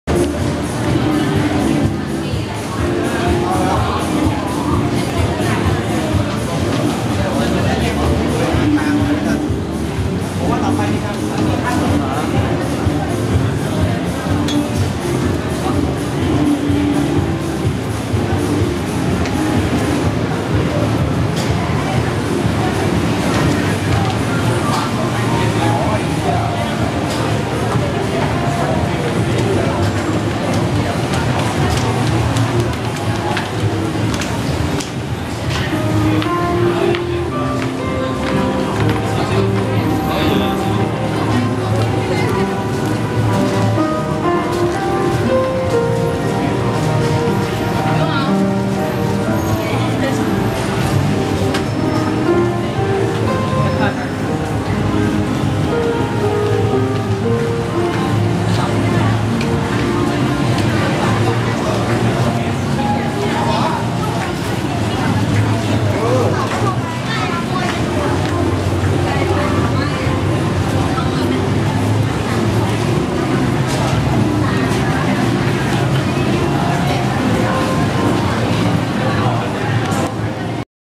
Walking from MBK to the National Stadium skytrain station in Bangkok
Walking from MBK to the skytrain in Bangkok